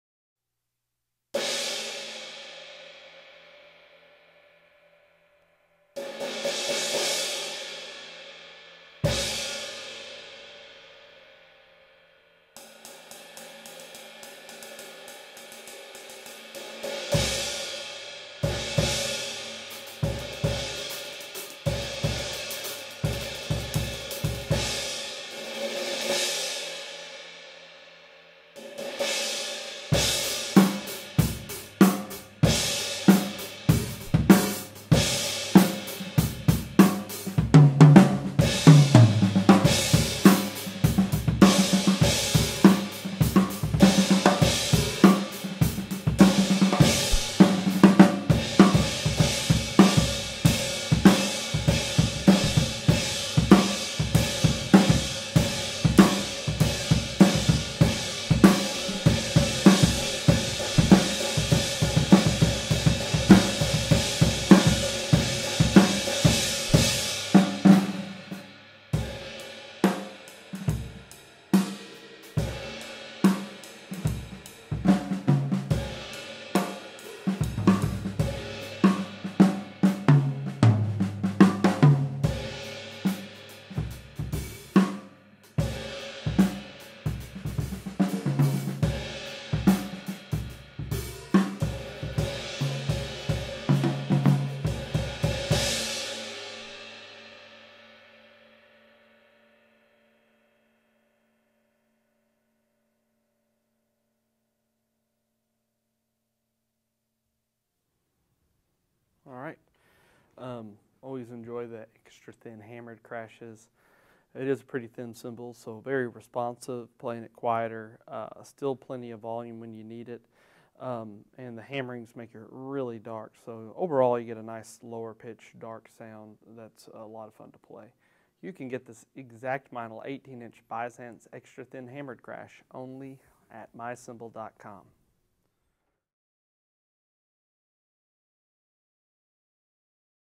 Meinl 18" Byzance Traditional Extra Thin Hammered Crash Cymbal - 1404g (B18ETHC-1111016L)
*SOLD* Meinl 18" Byzance Traditional Extra Thin Hammered Crash Cymbal available from Memphis Drum Shop.

Other gear used in this video:
• Cymbals (From drummers L to R): Meinl 15" Byzance Vintage Pure Hi Hat Cymbals, Meinl 22" Byzance Extra Dry Medium Ride Cymbal